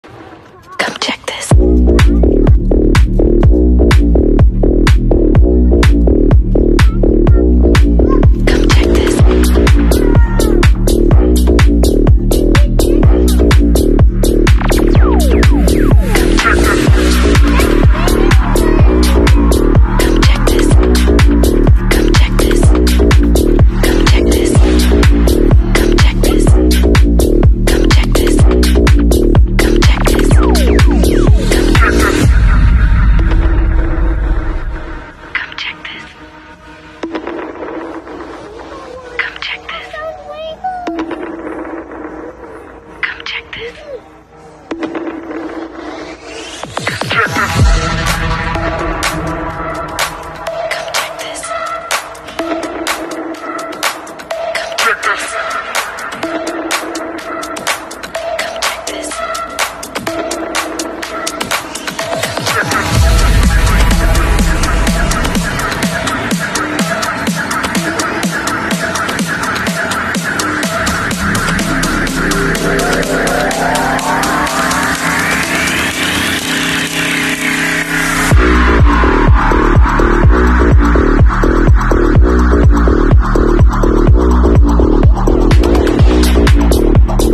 Dicks Sporting Goods
Where can you find all the summer items? @dickssportinggoods This store has a full line of summer items: sports activities, pool activities, shoes, clothes, and TOYS!!! Aria found her favorite Paw Patrol toys. COME CHECK THIS!